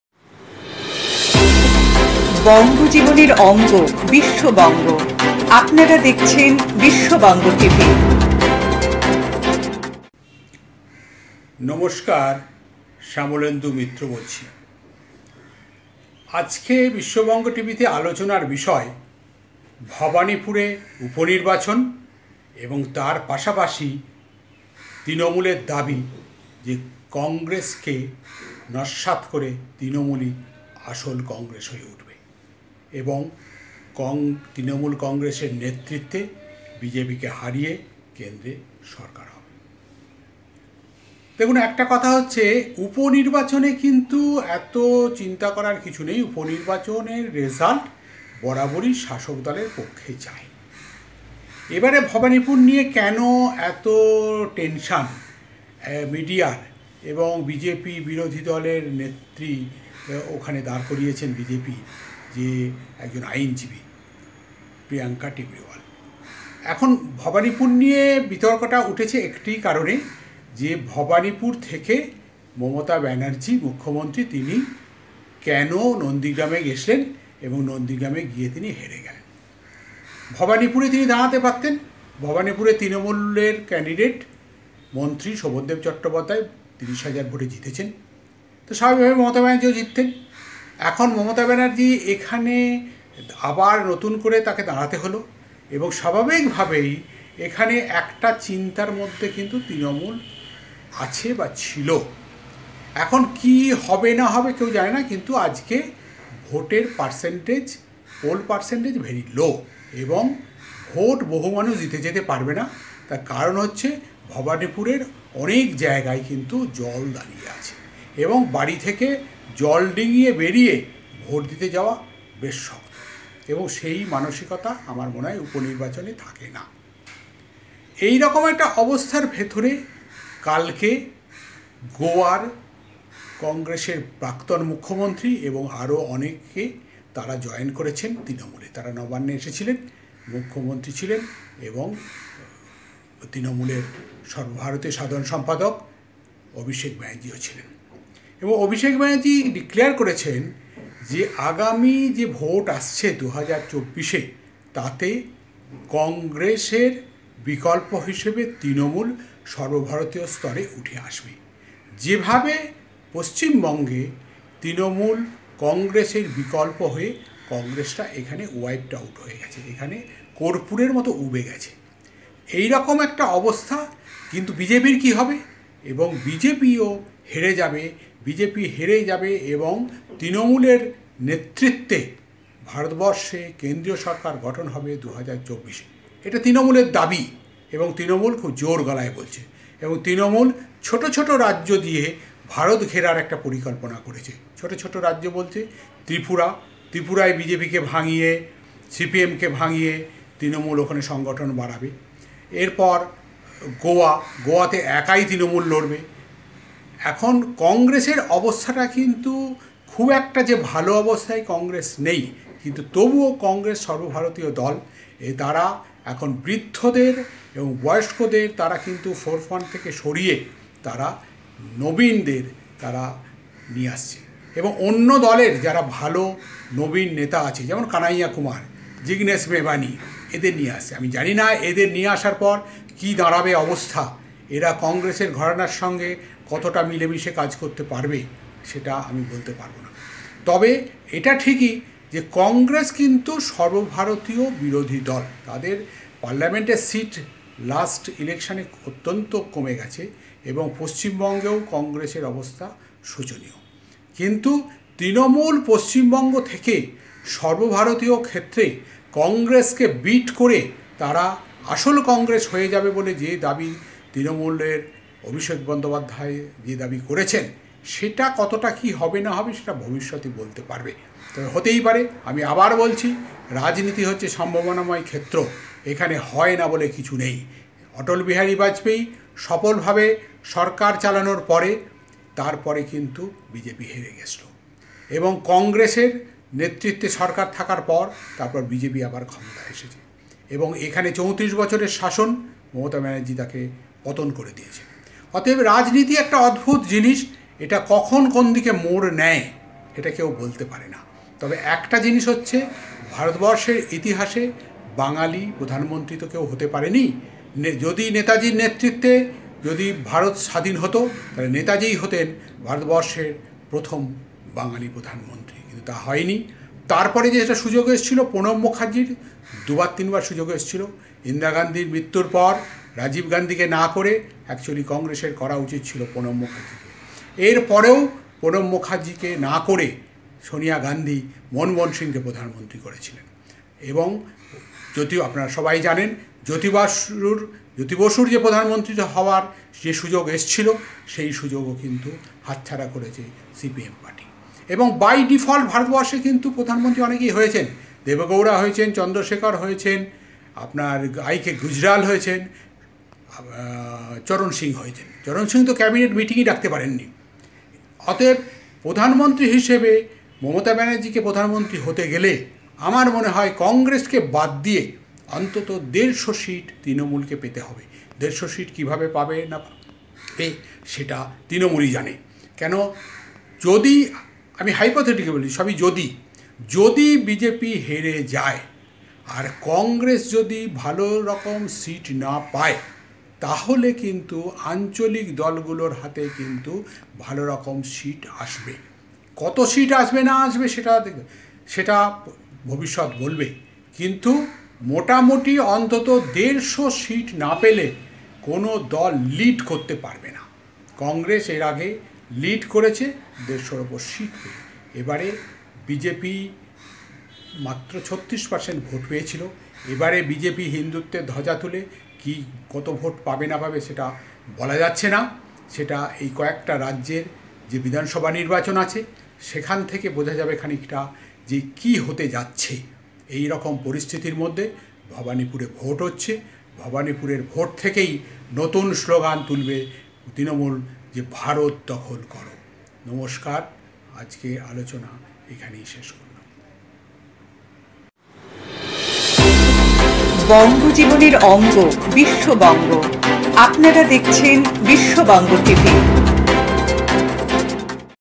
তৃণমূলই হবে দেশে আসল কংগ্রেস # বিজেপিকে হারিয়ে তৃণমূলের নেতৃত্বে কেন্দ্রে সরকার হবে # দাবি তৃণমূলের
তৃণমূল হবে আসল কংগ্রেস # বিজেপিকে হারিয়ে তৃণমূলের নেতৃত্বে কেন্দ্রে সরকার হবে # দাবি